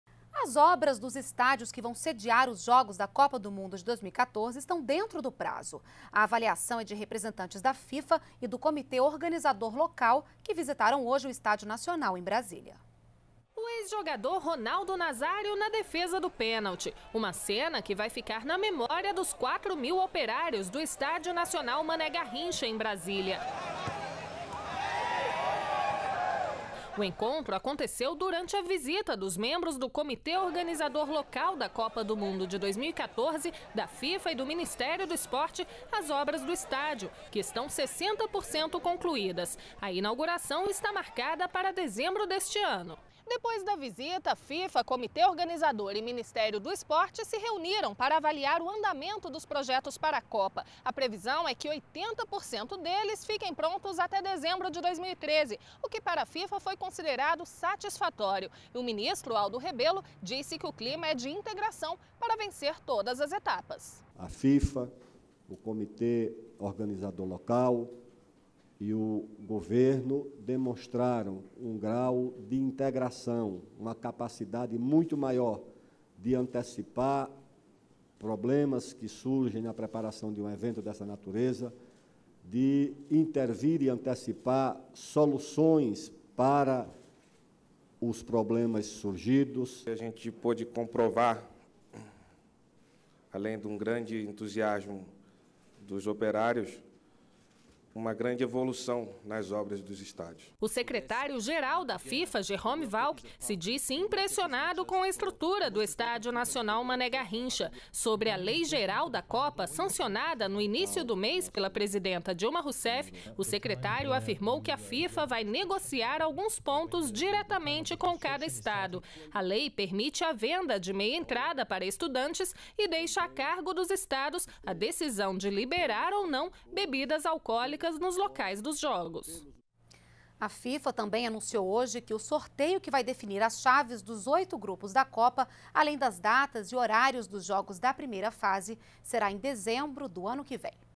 Obras dos estádios para Copa 2014 estão dentro do prazo
NBR NOTÍCIAS - 28.06.12: Representantes da Fifa e do Comitê Organizador local que visitaram nesta segunda-feira (28) o Estádio Nacional, em Brasília, avaliaram que as obras dos estádios que vão sediar os jogos da Copa de 2014 estão dentro do prazo.